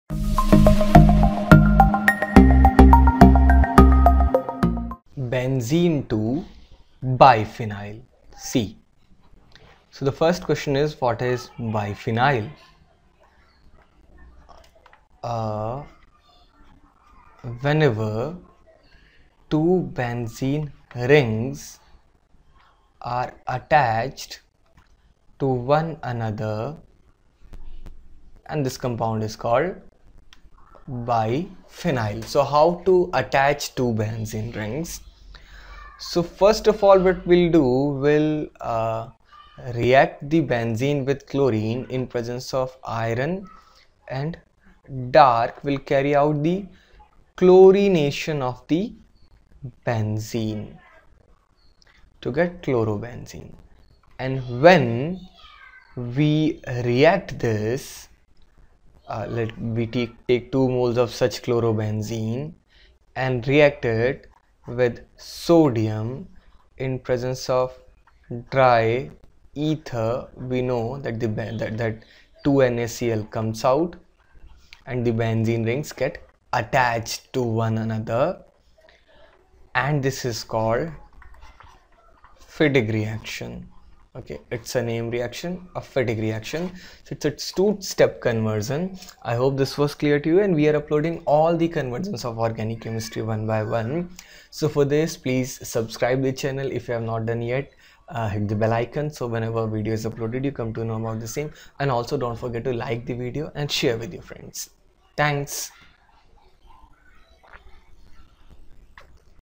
Benzene to Biphenyl |Organic chemistry conversions for JEE, NEET & CBSE
Convert Benzene to Bipheny. learn conversions of organic chemistry class 12 for iit jee, neet and cbse by chemistryconcept.com